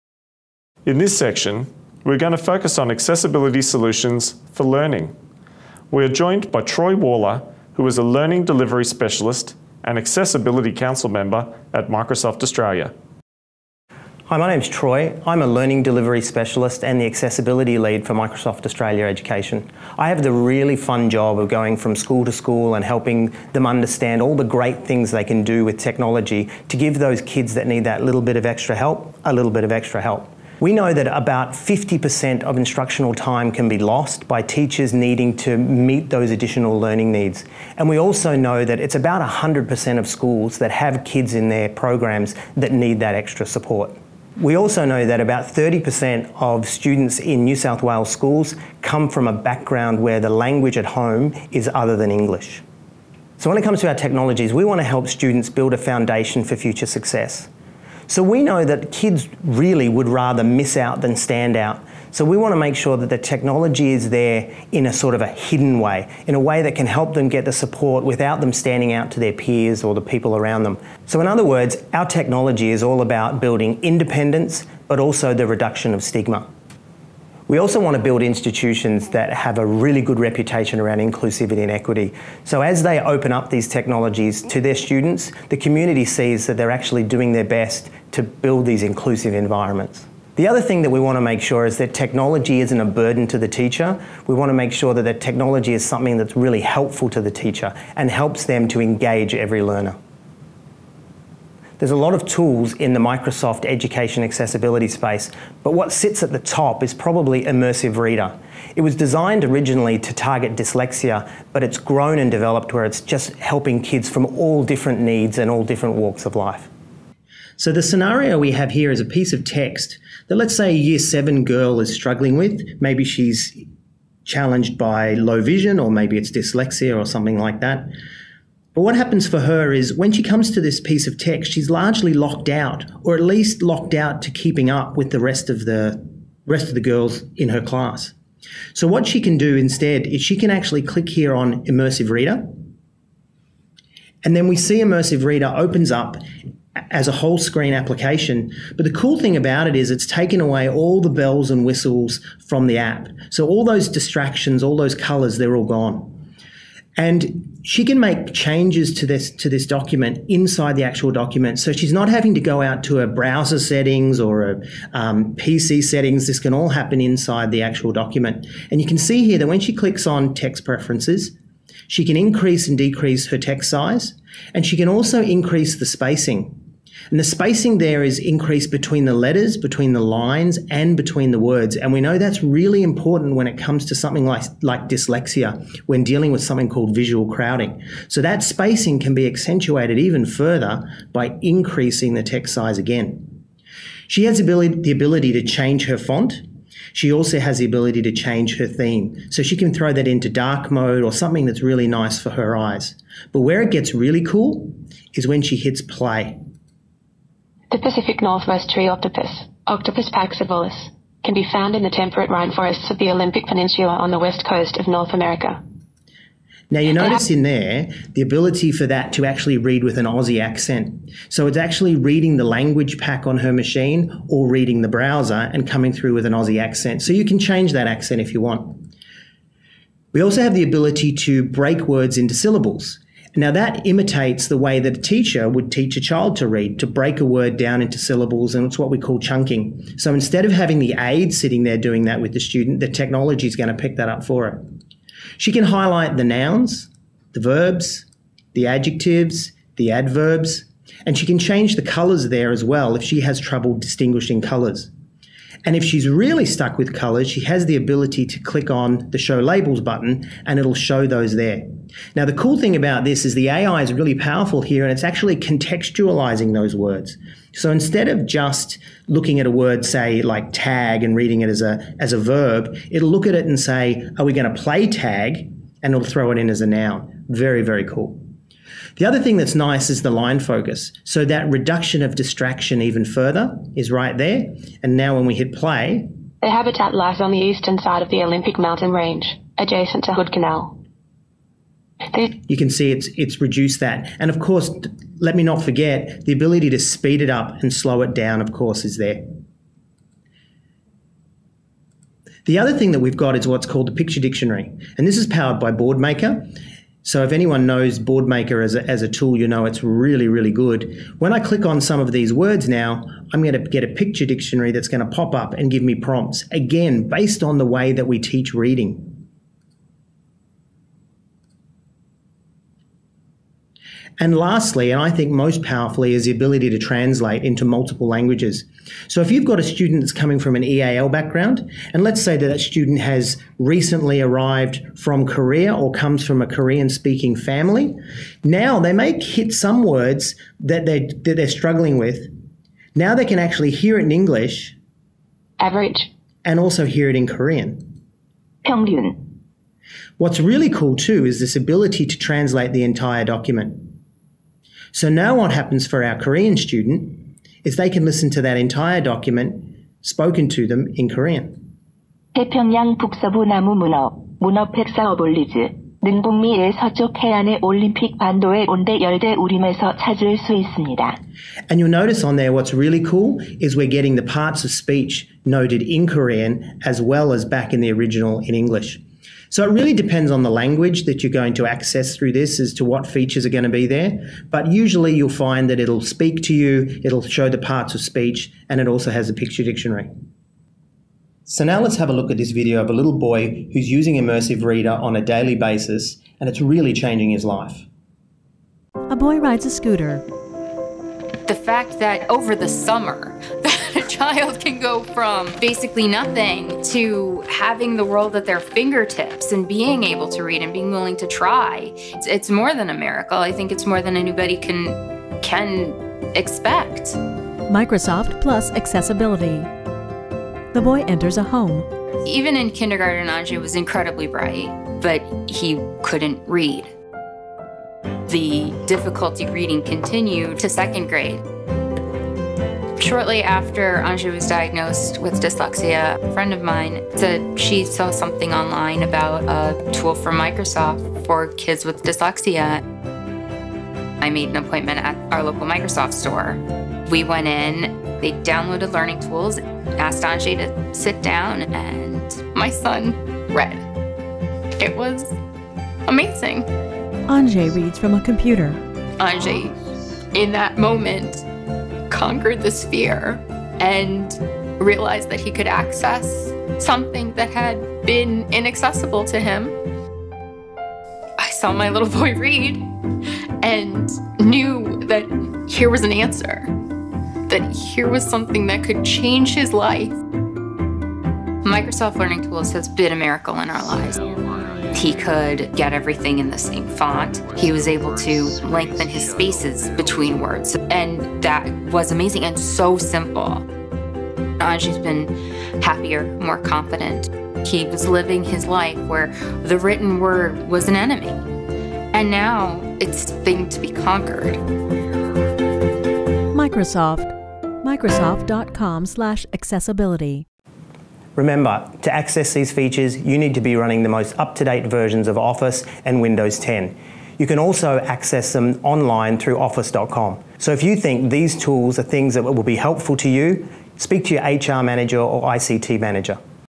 Introducing Immersive Reader
In this video, Troy introduces Immersive Reader. It is a free tool, built into Word, OneNote, Outlook, Office Lens, Microsoft Teams, Forms, Flipgrid, Minecraft Education Edition and the Edge browser, that implements proven techniques to improve reading and writing for people regardless of their age or ability.